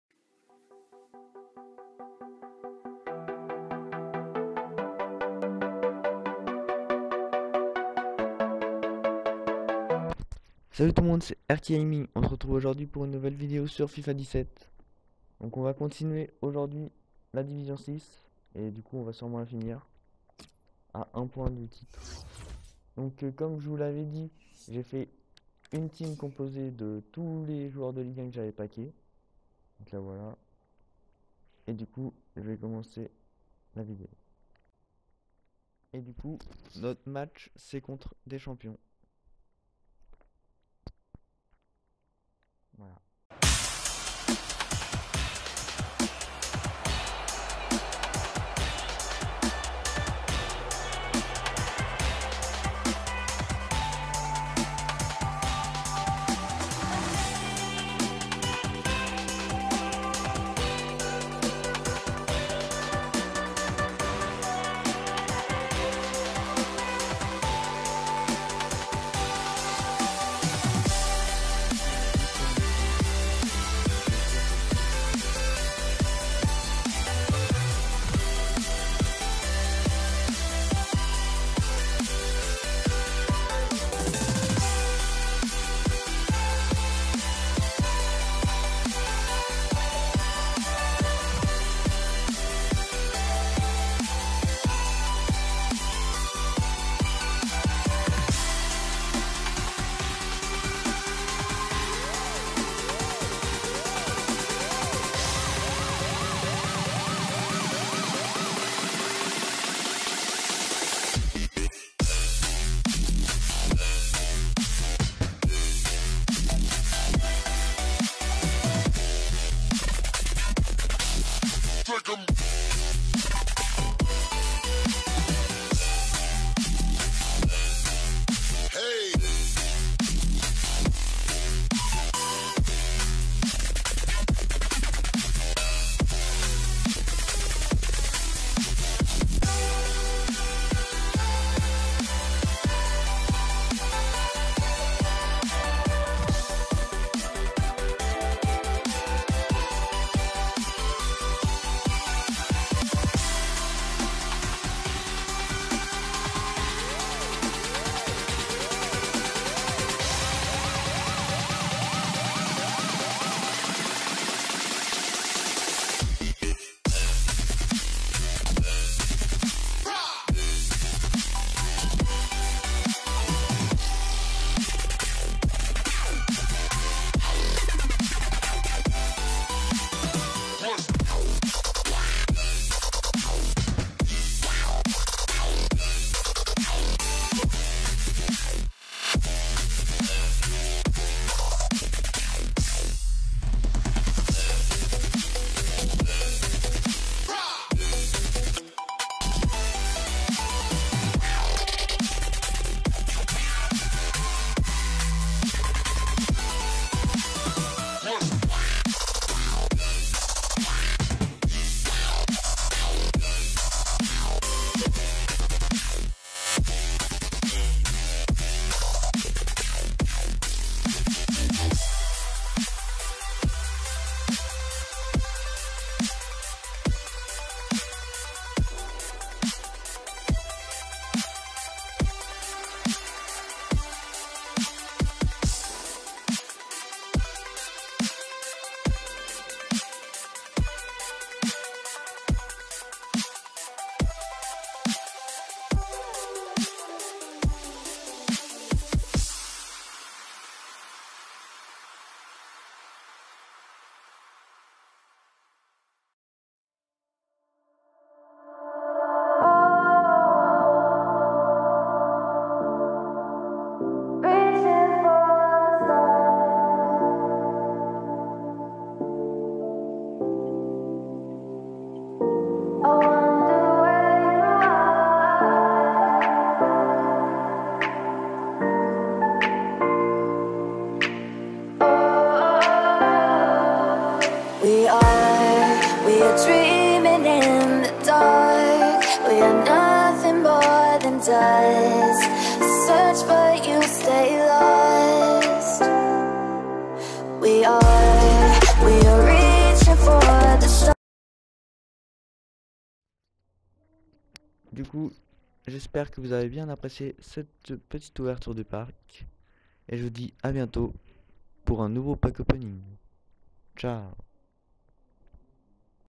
FIN DE DIVISION 6 ET QUELQUE PACK EN BONUS!!!
dans cette video je fini la divion 6 et j'ouvre quelque packs